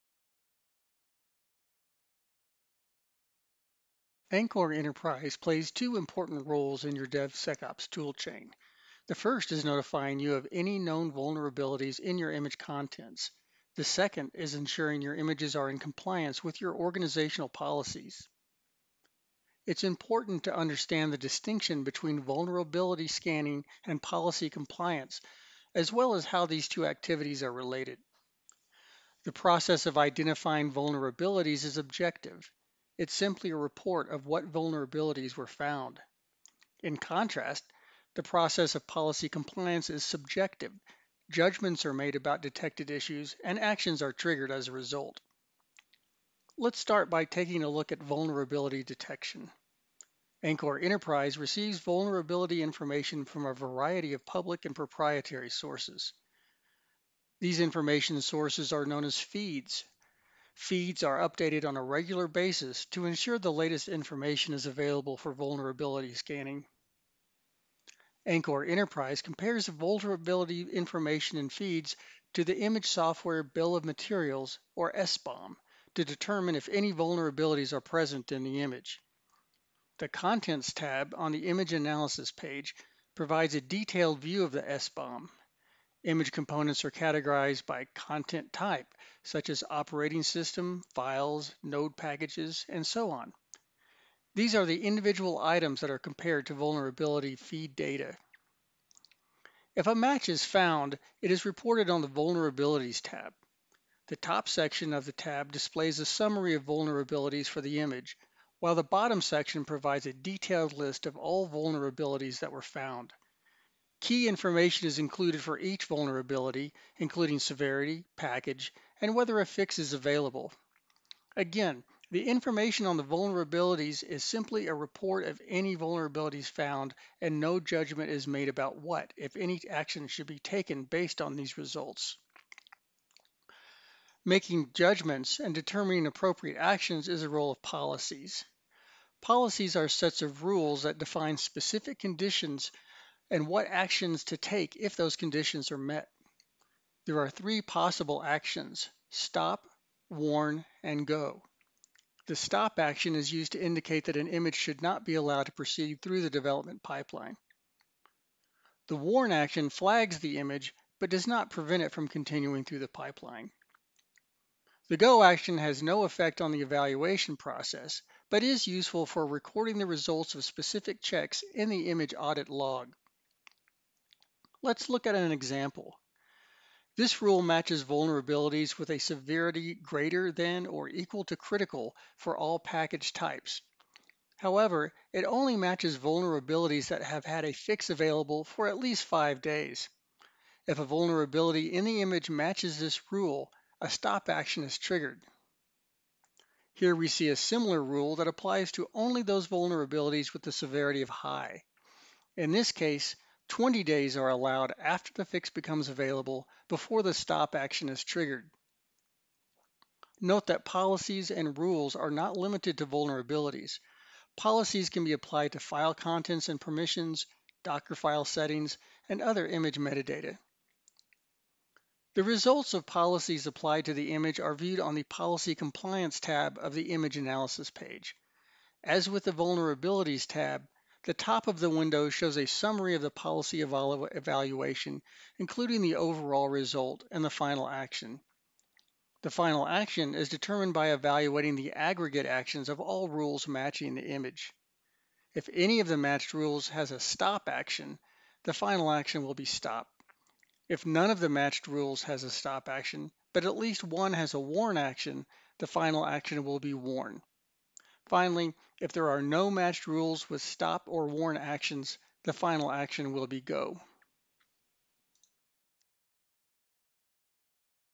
The DevSecOps Toolchain: Container Vulnerability and Policy Compliance with Anchore Enterprise
The Anchore Enterprise technology platform plays two important roles in the DevSecOps toolchain, that is to identify any known vulnerabilities in container image content and make sure container images are in compliance with your organization’s policies. These are two important elements of software supply chain security. This video explores the difference between vulnerability scanning and policy compliance through Anchore Enterprise. Kurt Huffman, Anchore senior technical content developer, discusses critical software supply chain security elements like a Software Bill of Materials, commonly referred to as SBOM security, along with commentary about when an image should not be allowed to continue through the software development pipeline.